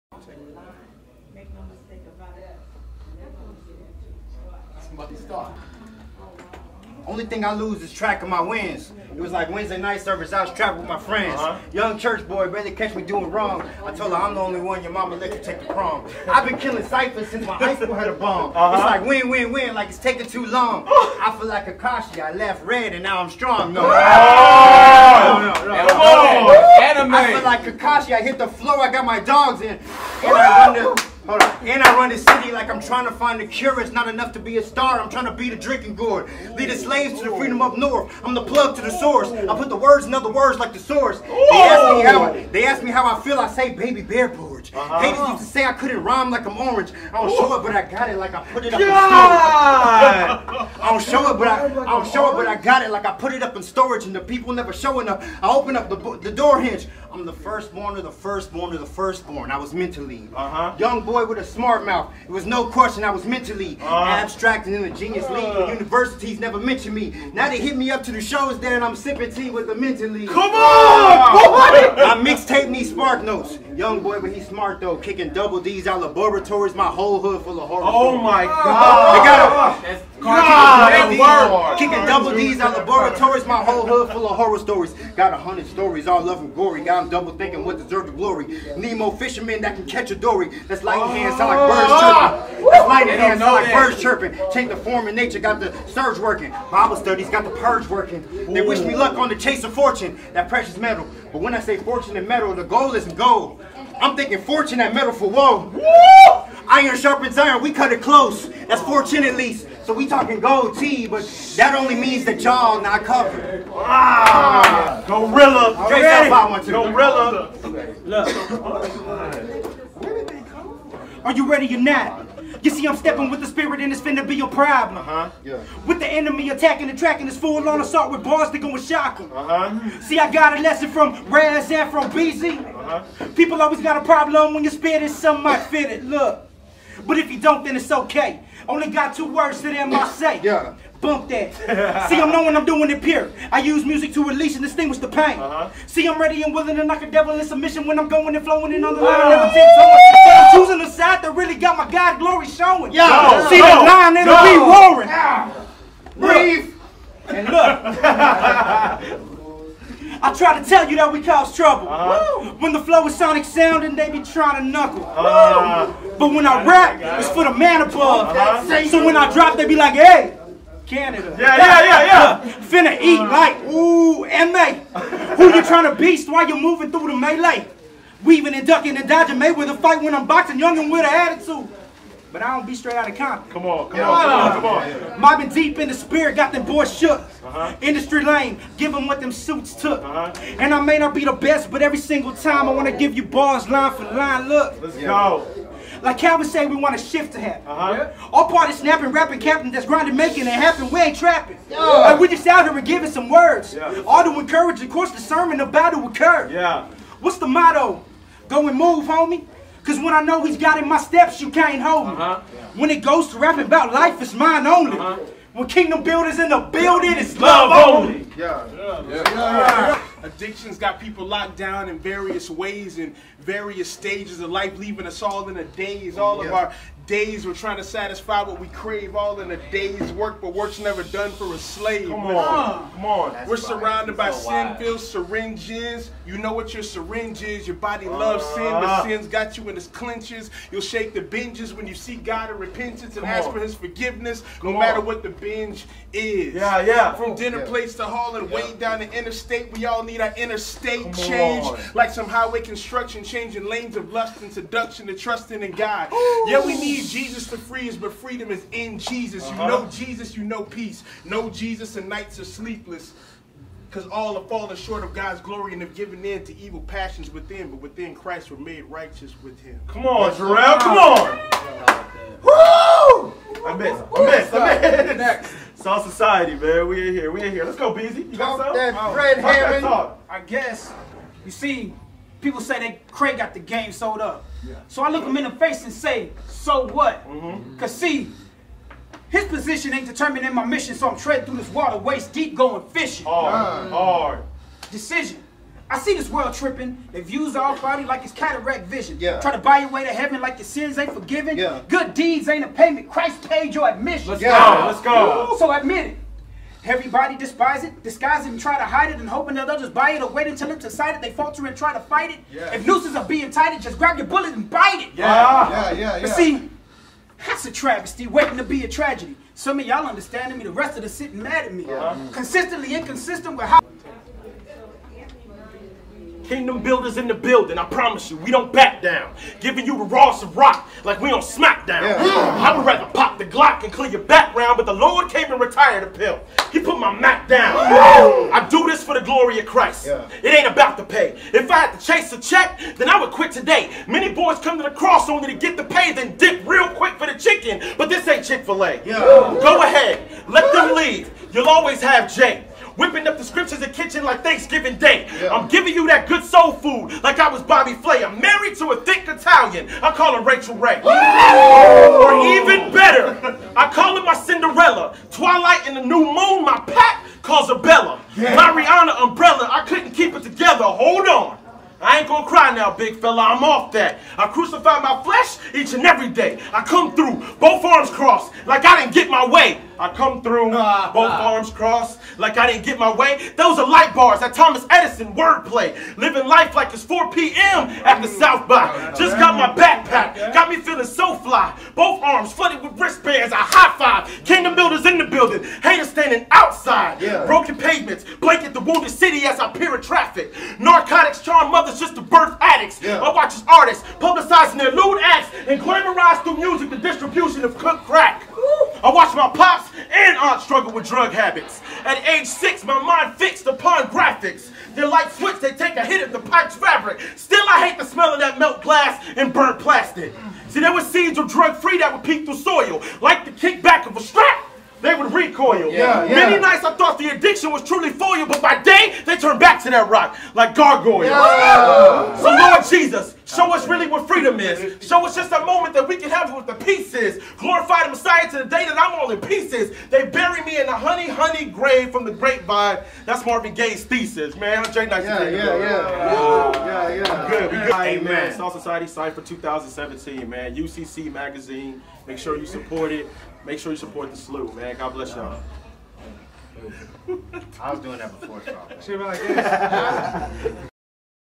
2017 SXSW Cypher at Kingdom Experience
Salt Society Cypher at Kingdom Experience 2017 hosted by UCC Magazine.